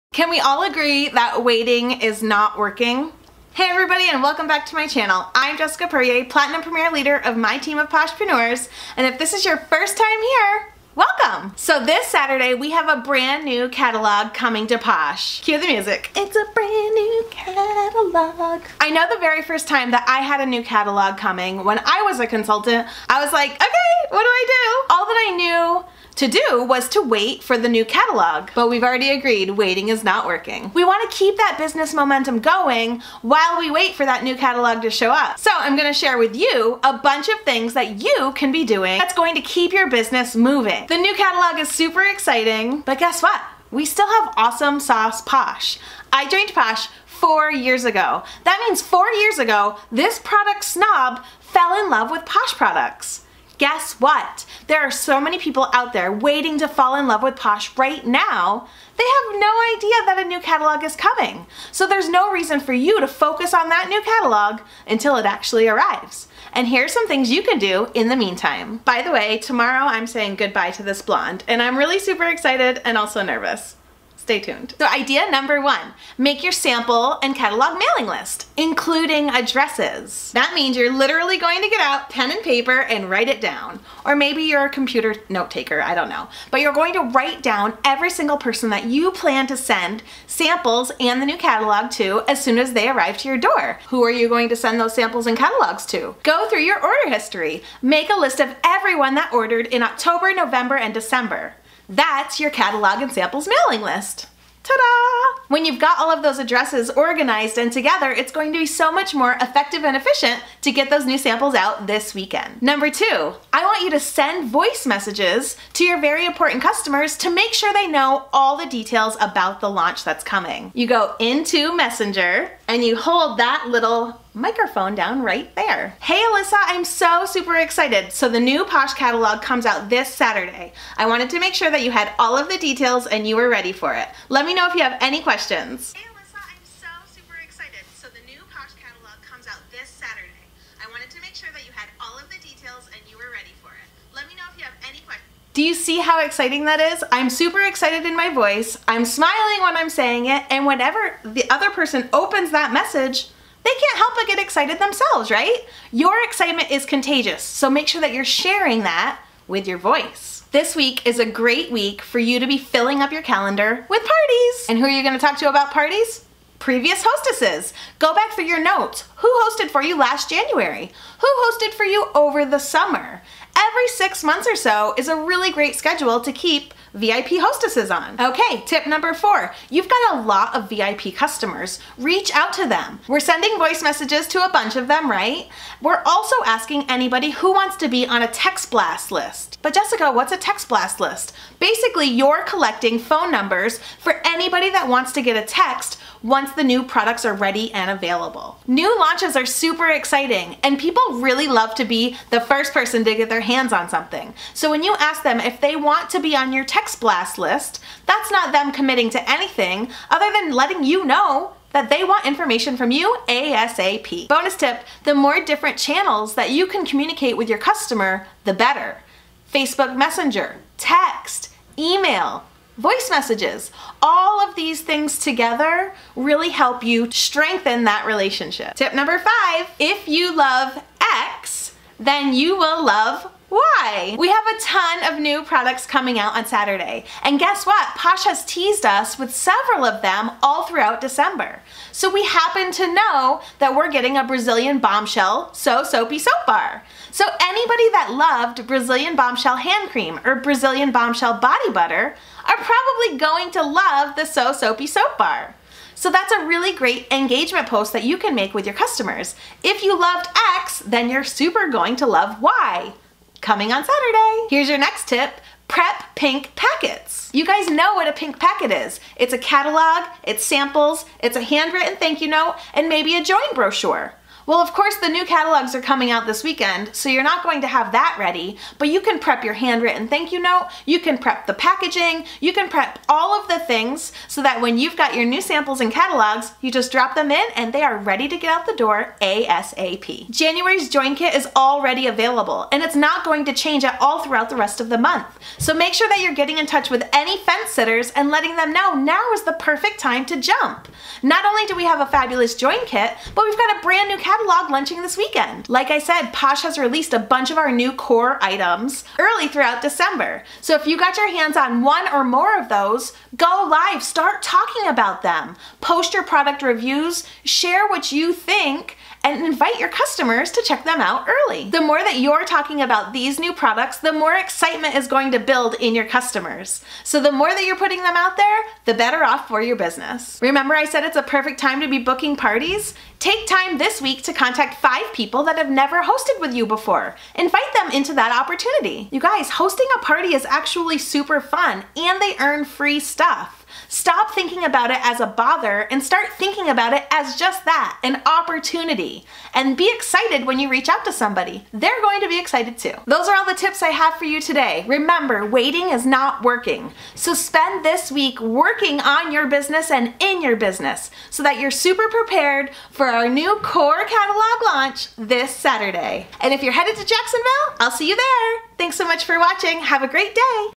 Preparing for a New Catalog Launch | Consultant Training - What to do
We have a new catalog launching this Saturday - but sitting around and waiting for it isn’t working toward our dreams!  Here are some things you can do this week to keep your momentum going in your Posh business.

Ready to become a Posh Consultant?!